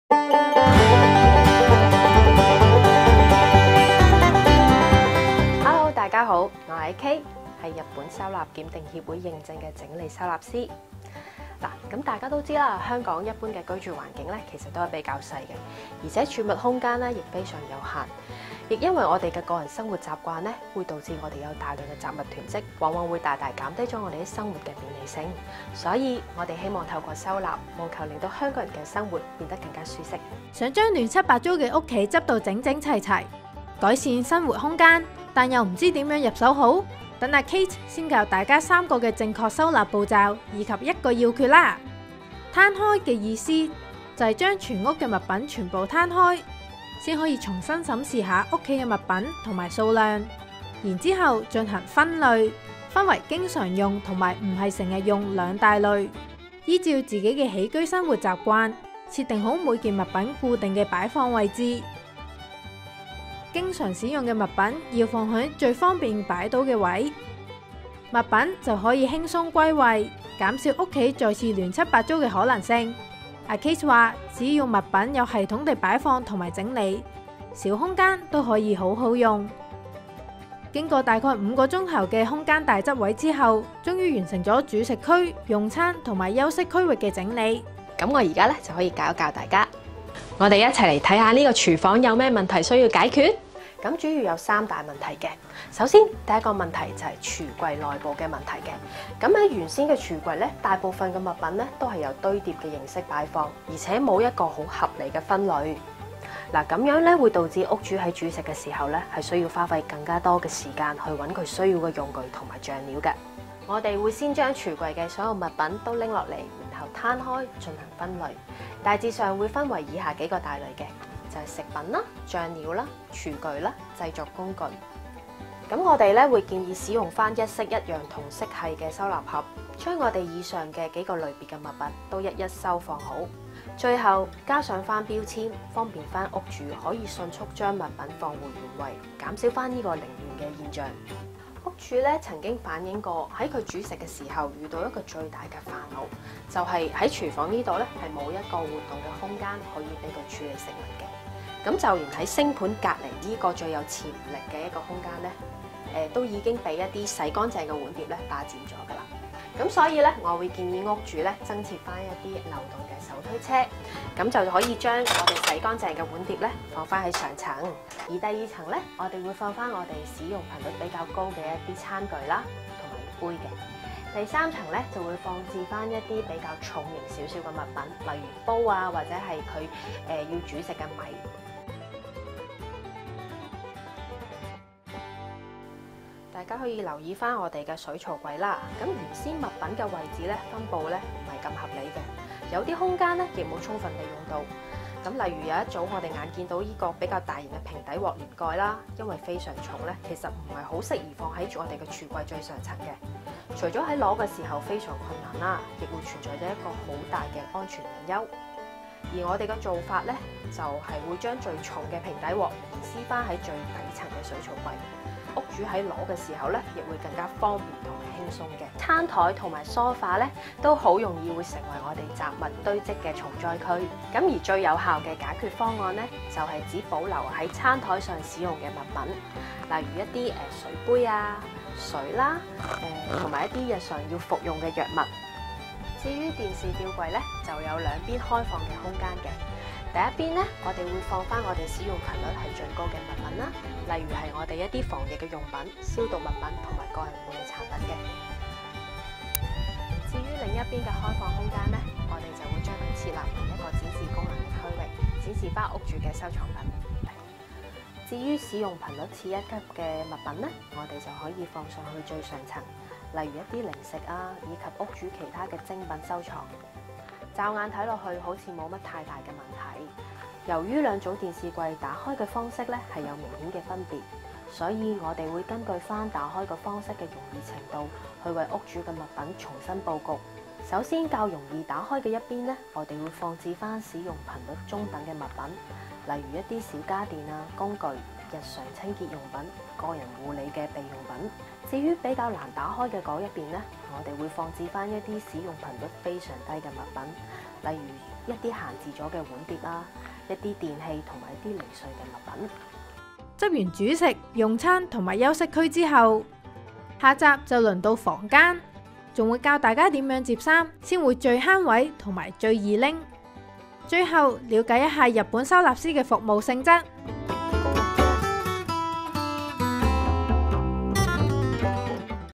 【收納達人教你執屋】日本認證收納師 教你3大家居收納步驟及要訣｜U Magazine生活頻道
香港一般嘅居住環境都比較細小，儲物空間有限，又因為生活習慣，好易囤積大量雜物。想將亂七八糟嘅屋企，執得整整齊齊，又唔知從何入手？今次搵咗 日本收納檢定協會認證收納師，教大家 正確收納步驟 及 收納要訣。其實小空間，都可以好好用。

【info】
日本收納檢定協會認證收納師
more than TIDYUP

收納用具詳情：
仲縮棍
價錢：12 港元
購買點：AEON 12 蚊店

文件架
價錢：12港元

白色收納盒
價錢：19.9 港元
購買點： IKEA
全方位緊貼《U Magazine》社交平台